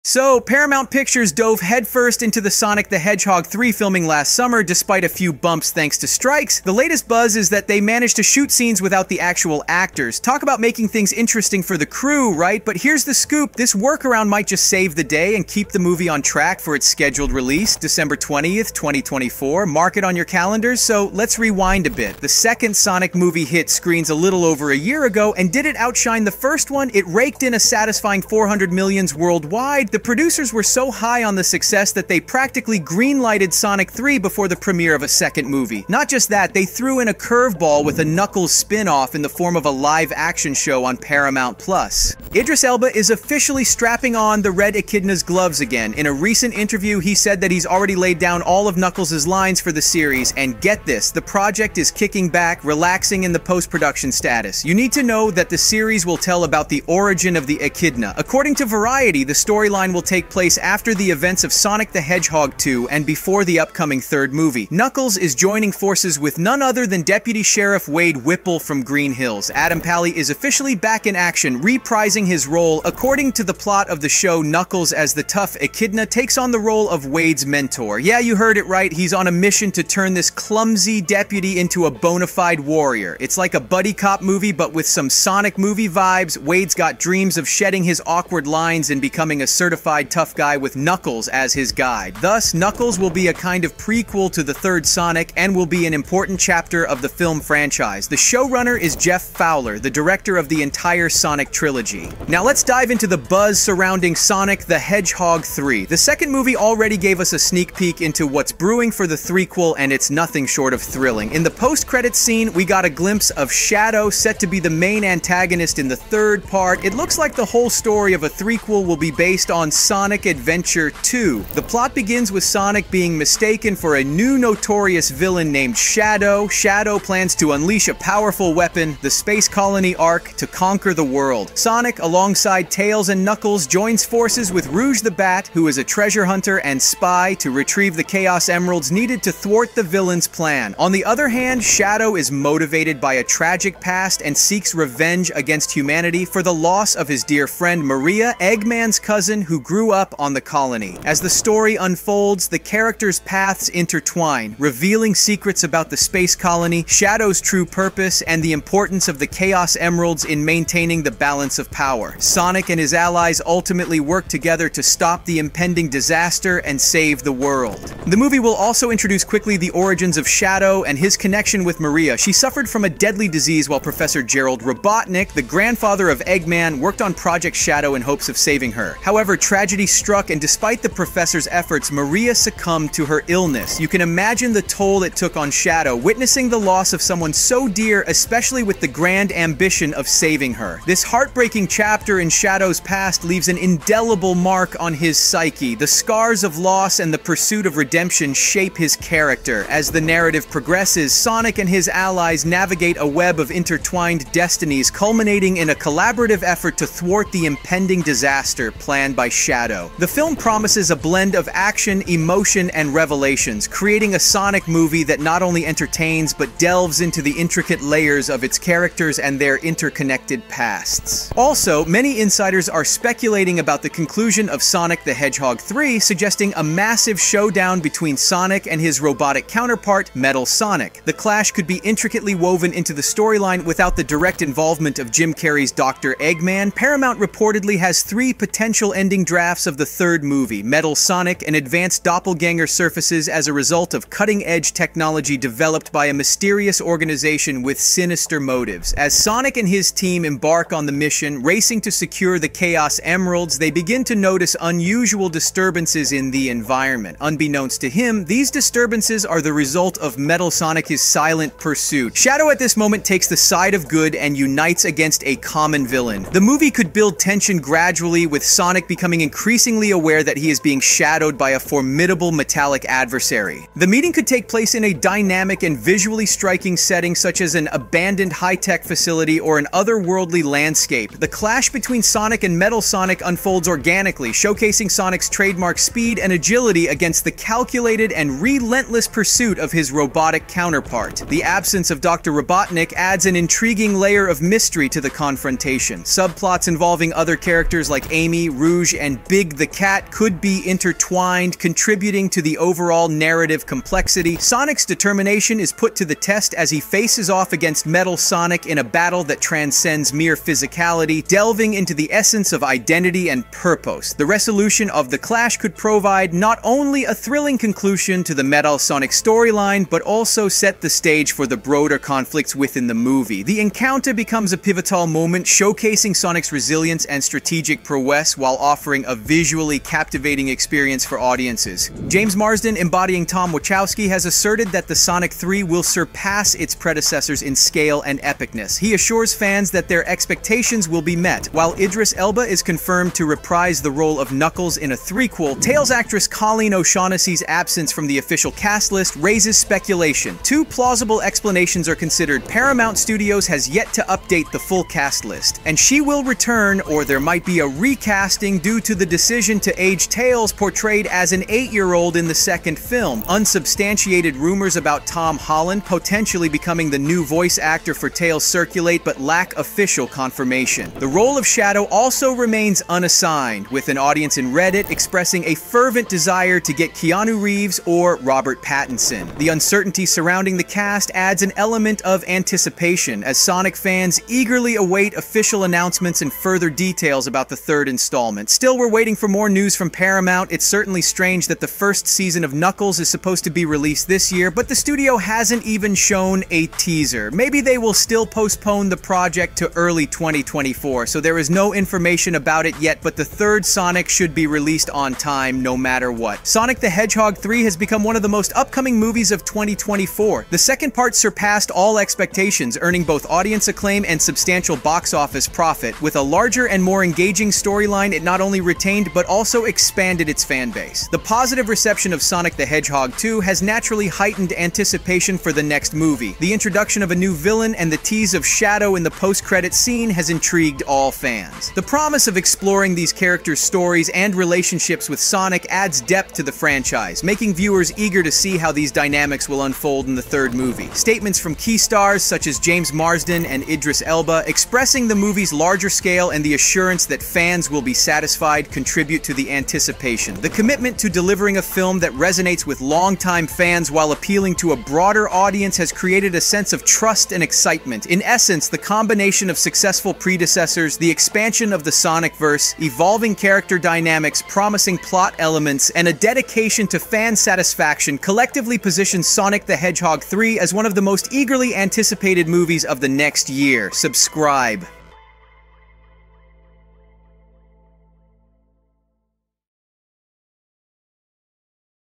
Sonic the Hedgehog 3 – Teaser (2024) Official Update
Paramount Pictures has officially resumed production of Sonic the Hedgehog 3 movie. Director Jeff Fowler announced this. The actors announced their return to roles. The story features Shadow, who was officially teased in the post-credits scene of Sonic the Hedgehog 2, and the possible appearance of Metal Sonic.

00:00 Movie Status
00:37 Knuckles Spin-Off
01:42 Shadow & Rogue
03:41 Metal Sonic
05:31 Cast
06:29 Official Update